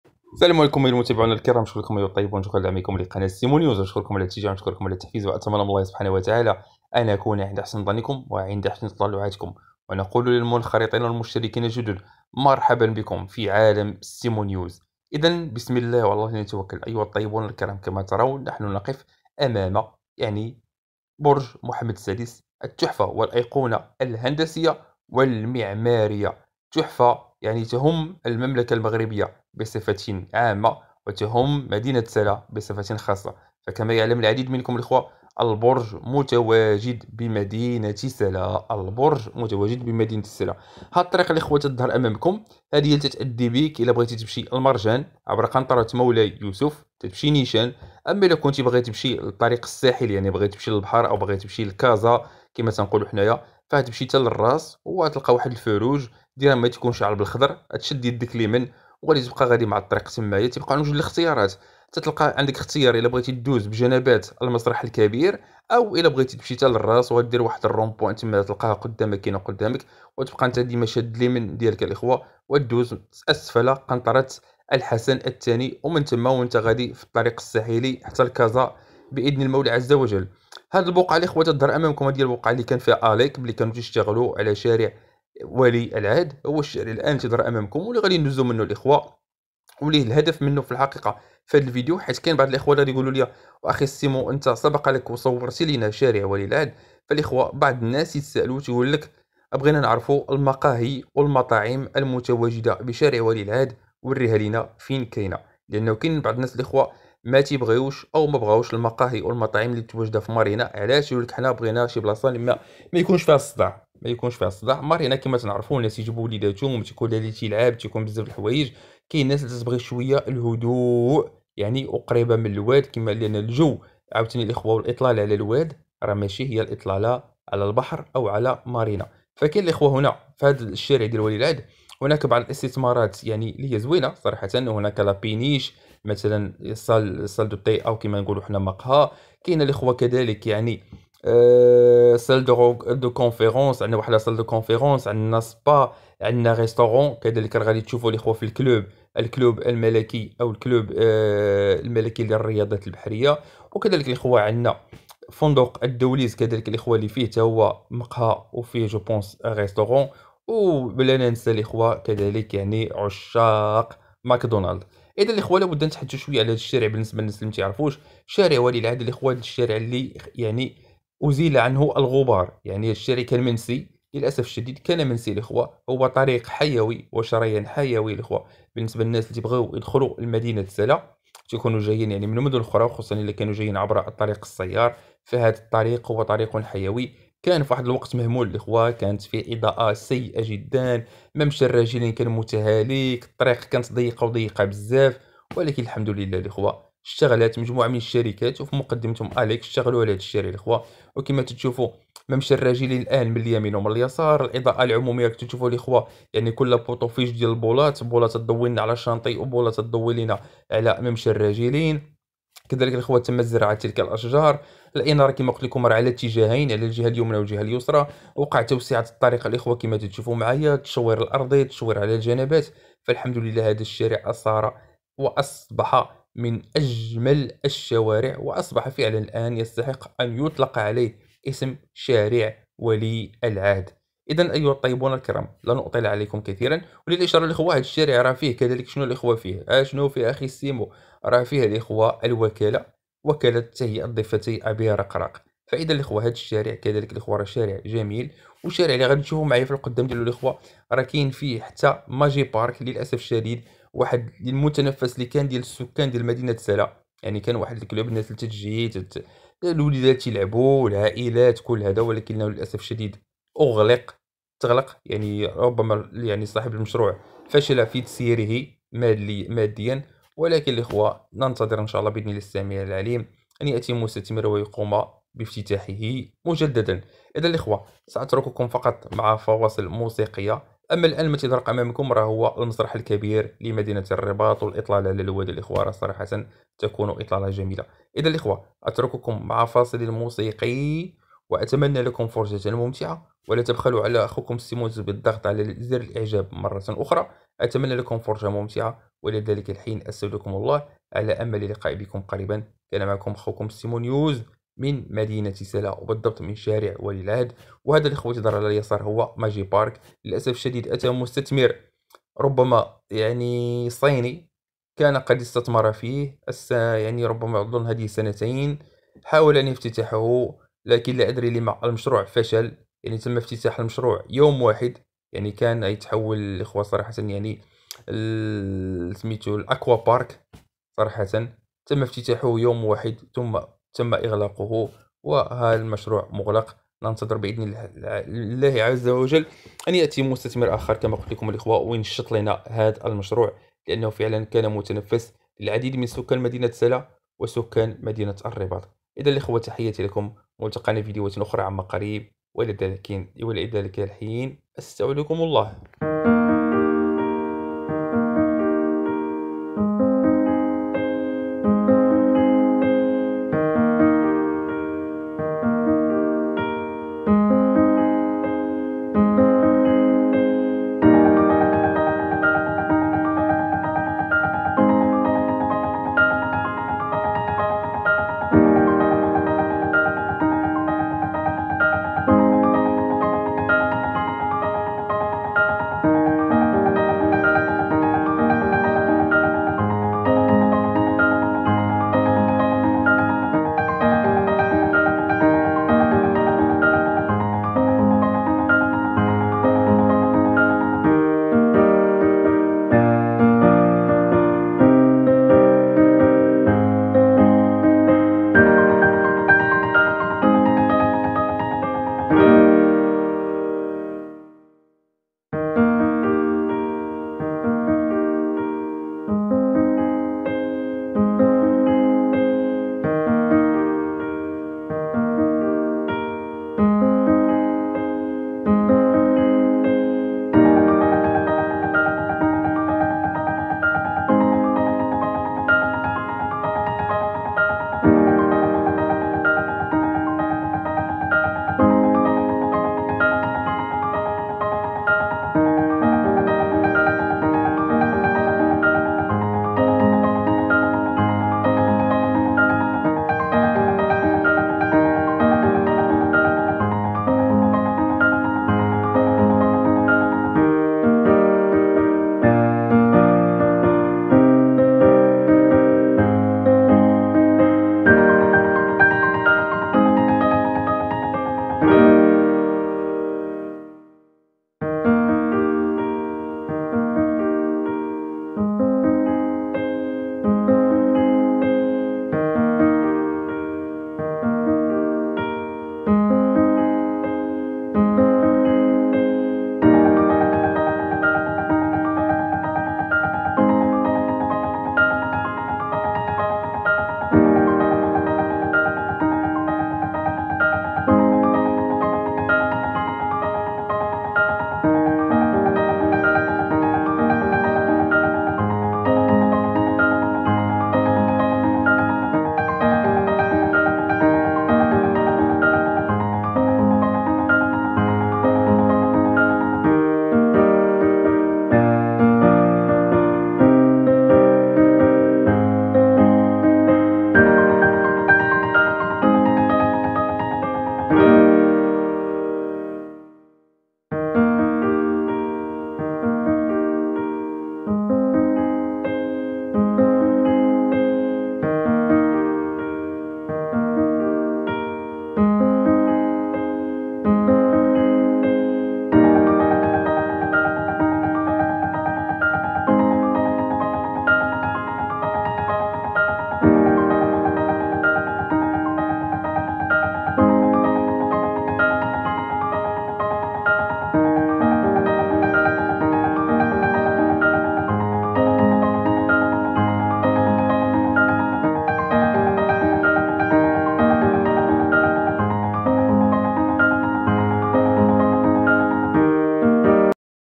برج محمد السادس جولة من البرج في اتجاه ماكدونالد مرورا بشارع ولي العهد
يُعد شارع ولي العهد أحد أهم الشوارع في سلا شار معروف و هو يضم مقاهي و مطاعم و كدلك  مقر وكالة تهيئة ضفتي أبي رقراق عرف تحول جدريا بعد مرور الأشغال